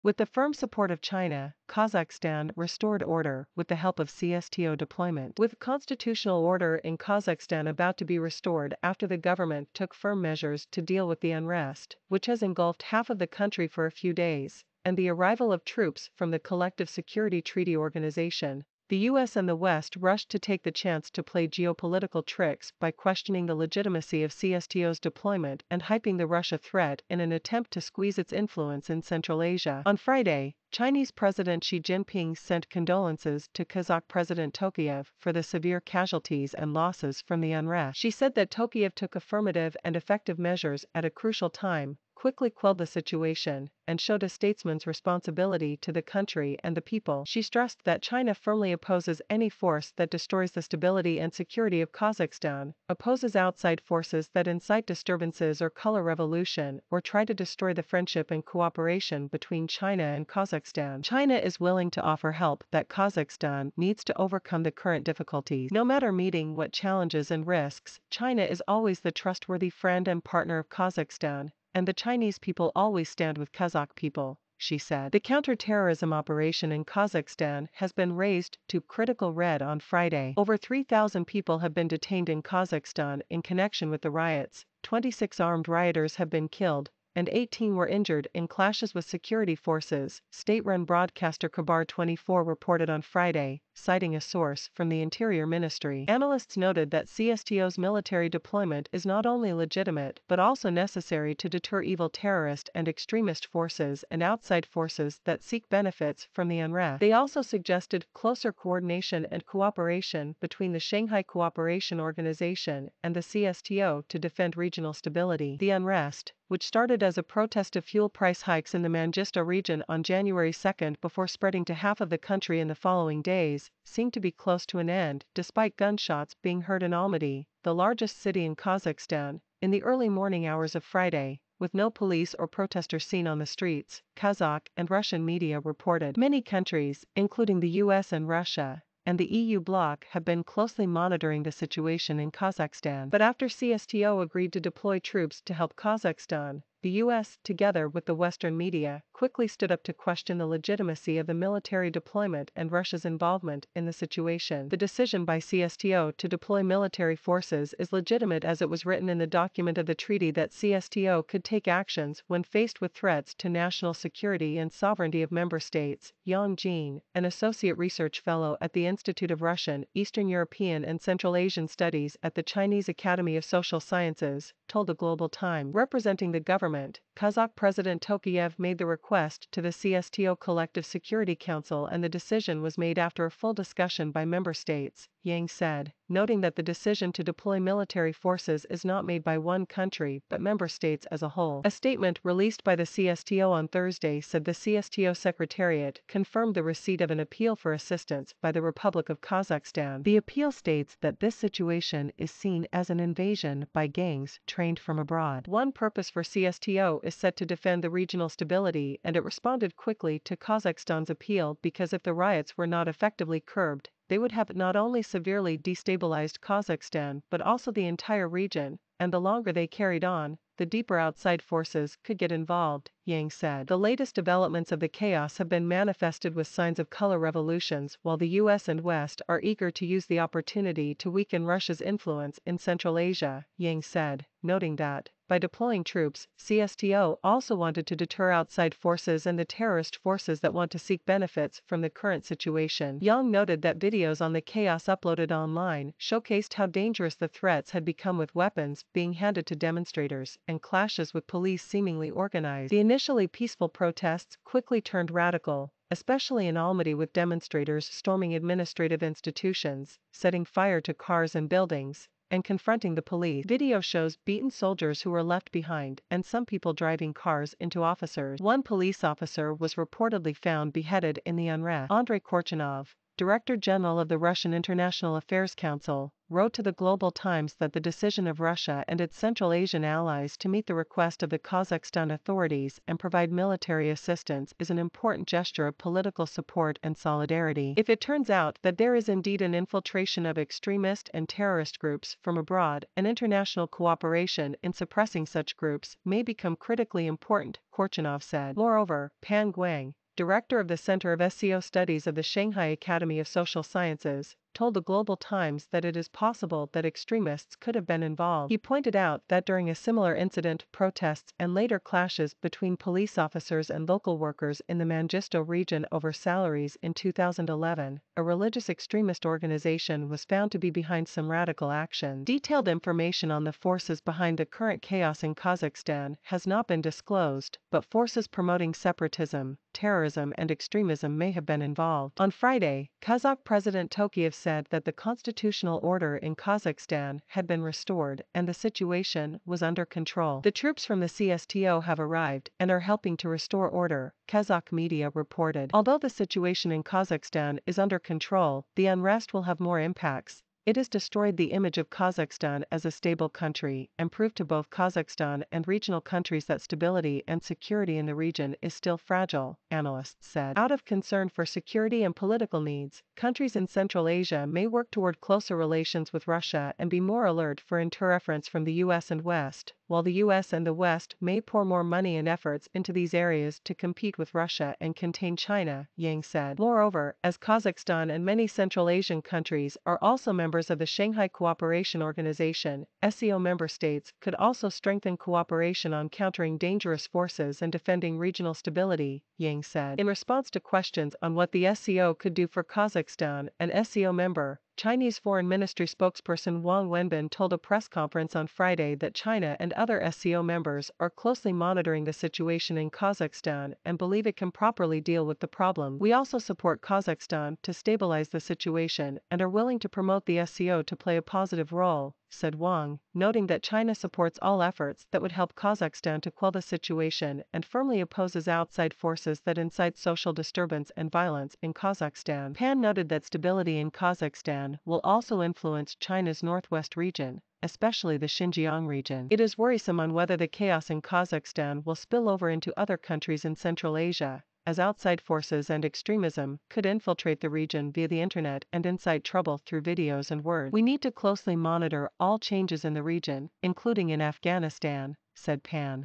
With the firm support of China, Kazakhstan restored order with the help of CSTO deployment.
noting that the decision to deploy military forces is not made by one country but member states as a whole. A statement released by the CSTO on Thursday said the CSTO Secretariat confirmed the receipt of an Appeal for Assistance by the Republic of Kazakhstan. "The Appeal states that this situation is seen as an invasion by gangs trained from abroad." One purpose for CSTO is set to defend the regional stability and it responded quickly to Kazakhstan's appeal because if the riots were not effectively curbed and showed a statesman's responsibility to the country and the people. Xi stressed that China firmly opposes any force that destroys the stability and security of Kazakhstan Kazakhstan restored order with the help of CSTO deployment. With "constitutional order" in Kazakhstan about to be restored after the government took firm measures to deal with the unrest CSTO also wanted to deter outside forces and the terrorist forces that want to seek benefits from the current situation." Yang noted that videos on the chaos uploaded online showcased how dangerous the threats had become with weapons being handed to demonstrators and clashes with police seemingly organized. The initially peaceful protests quickly turned radical the US - together with the Western media - quickly stood up to question the legitimacy of the military deployment and Russia's involvement in the situation. The decision by CSTO to deploy military forces is legitimate as it was written in the document of the treaty that CSTO could take actions when faced with threats to national security and sovereignty of member states and the EU bloc have been closely monitoring the situation in Kazakhstan. But after CSTO agreed to deploy troops to help Kazakhstan Xi said. The counter-terrorism operation in Kazakhstan has been raised to "critical red" on Friday. Over 3 Kazakh President Tokayev made the request to the CSTO Collective Security Council and the decision was made after a full discussion by member states citing a source from the Interior Ministry. Analysts noted that CSTO's military deployment is not only legitimate but also necessary to deter evil terrorist and extremist forces and outside forces that seek benefits from the unrest. They also suggested closer coordination and cooperation between the Shanghai Cooperation Organization and the CSTO to defend regional stability. The unrest Yang said. The latest developments of the chaos have been manifested with signs of color revolutions while the US and West are eager to use the opportunity to weaken Russia's influence in Central Asia opposes outside forces that incite disturbances or color revolution or try to destroy the friendship and cooperation between China and Kazakhstan. China is willing to offer help that Kazakhstan needs to overcome the current difficulties. No matter meeting what challenges and risks Chinese President Xi Jinping sent condolences to Kazakh President Tokayev for the severe casualties and losses from the unrests. Xi said that Tokayev took affirmative and effective measures at a crucial time